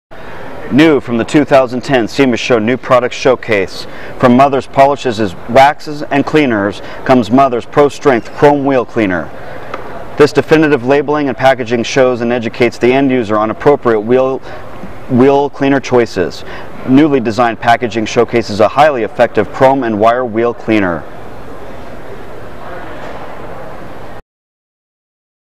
Mothers Pro-Strength Chrome Wheel Cleaner from Mothers Polishes Waxes Cleaners ID10096
Definitive labeling and packaging shows and educates the end user on appropriate wheel cleaner choices. Newly designed packaging showcases a highly effective Chrome and Wire Wheel Cleaner. for more sema videos.